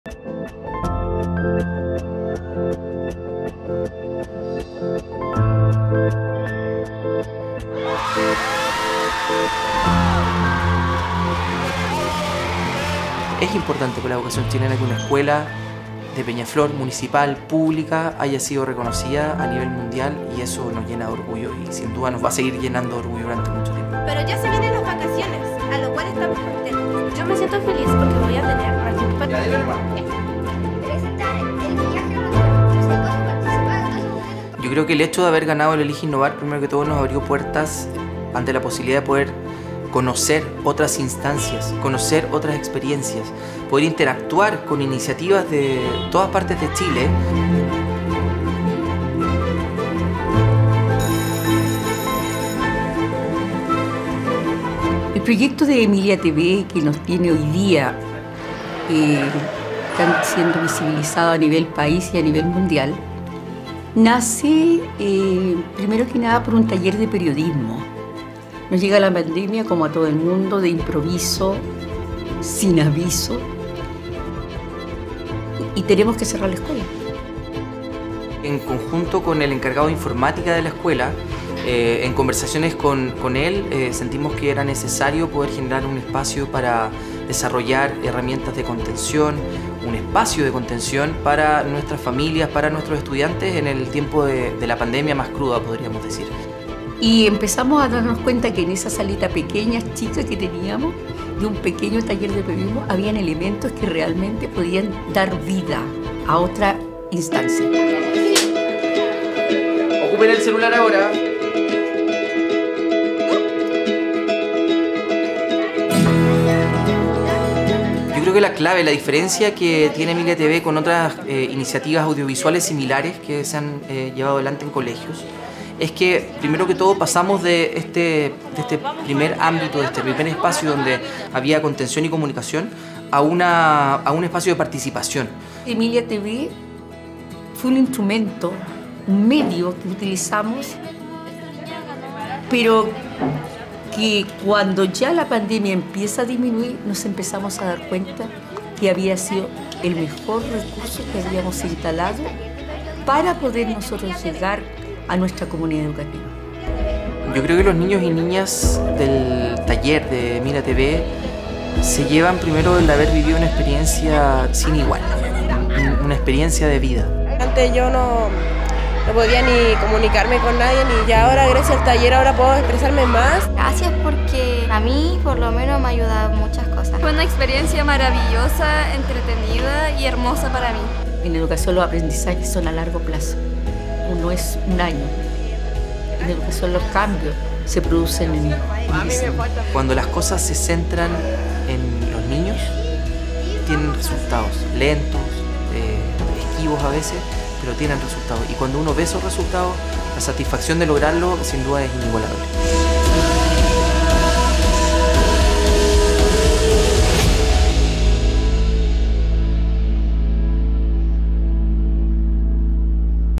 La historia de Emilia Lascar
Emilia TV fue un proyecto que nació en plana panademia para conectar a la comunidad de la escuela Emilia Lascar de Peñaflor. Hoy, le valió a ese establecimiento ser reconocidos como la escuela más innovadora del mundo en los premios World's Best School. Esta es su historia.